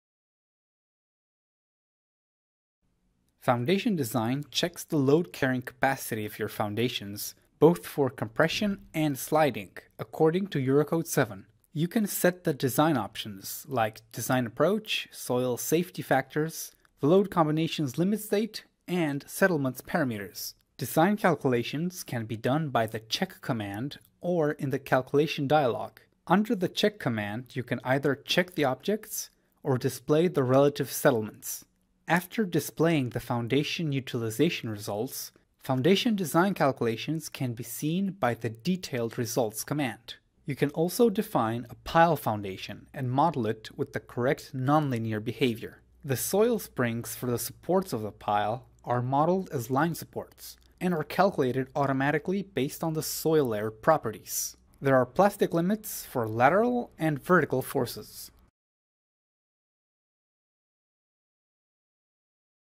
Geotechnics - Foundation Design
The Foundation Design module in FEM-Design helps you perform geotechnical checks of complete 3D structures, or just frames. The study and modification of soil and rocks should be a top priority when you run complex structural designs and want to assure sustainability, stability and safety and to make sure that even the most detailed analysis is being taken care of to prevent eventual negative construction outcomes. Watch the quick overview video.

Read more about the Foundation Design theory on the FEM-Design Wiki